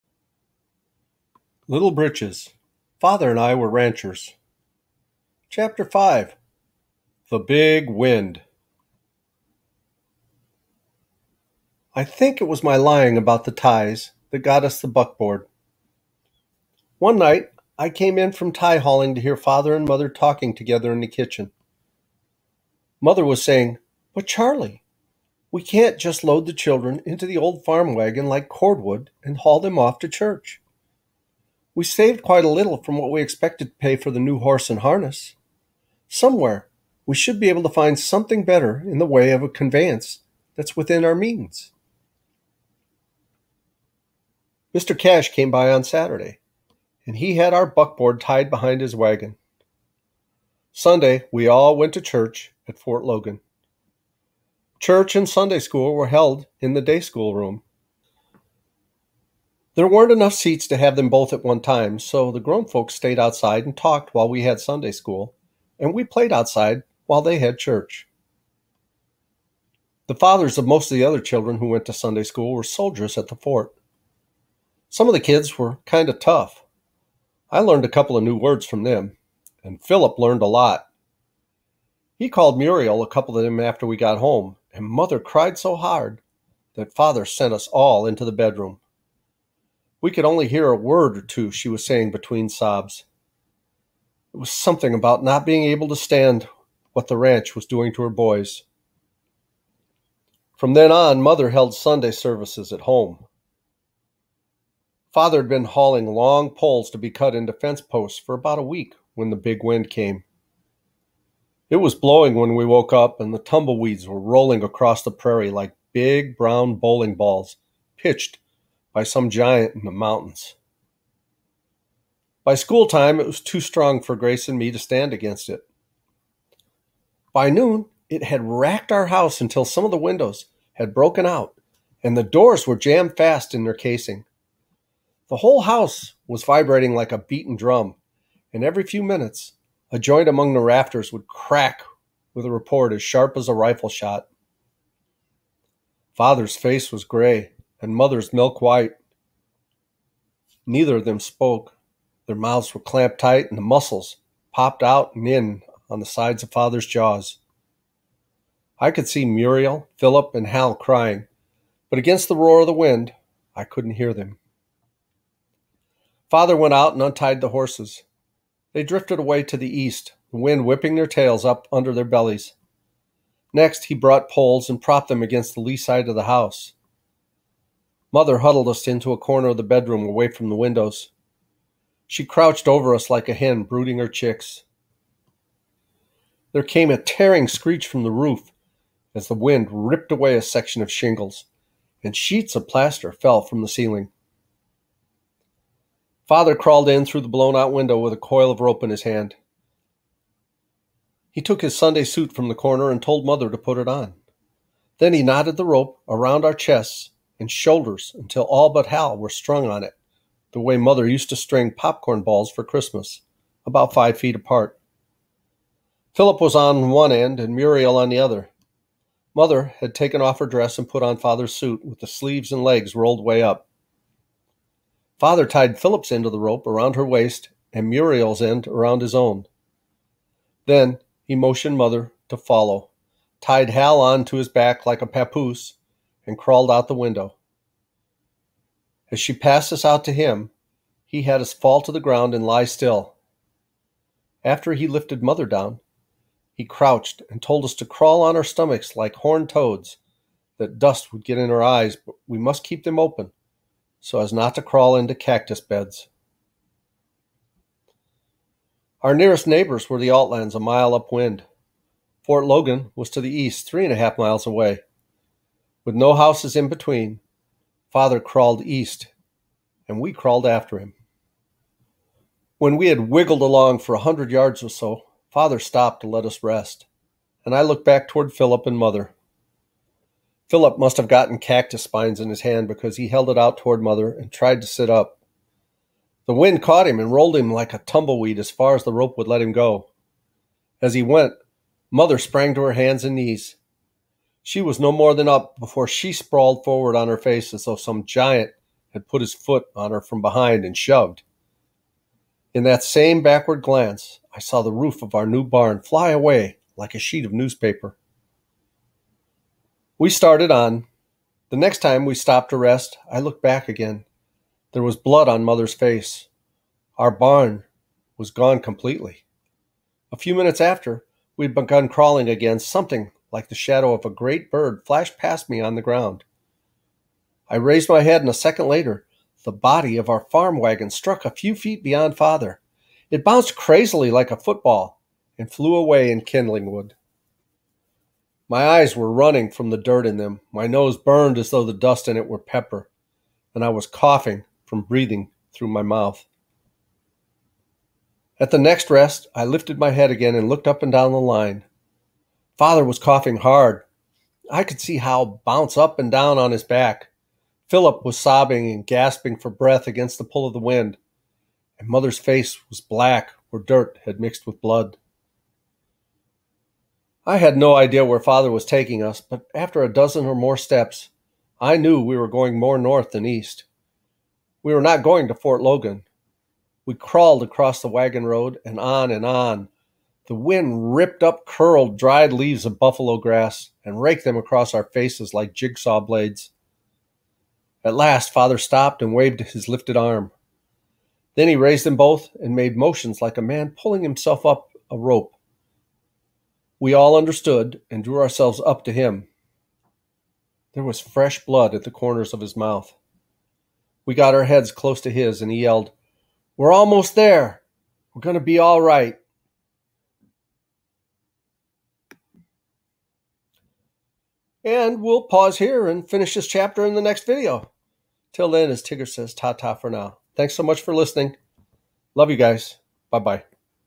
Little Britches chapter 5a | CC Challenge | audio book | Ralph Moody
Little Britches Father and I were Ranchers. CC Challenge audio book by Ralph Moody.